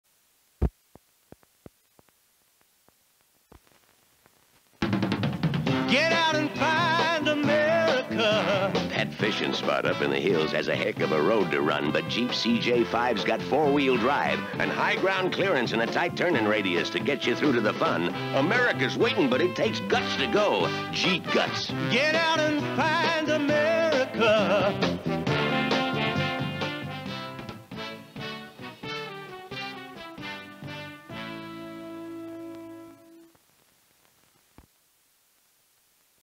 1975 AMC Jeep CJ5 Commercial - Toughest Four Letter Word
This is an AMC Jeep Dealer's commercial.   Uses a different advertising agency from the AMC corporate agency.   The toughest four letter word.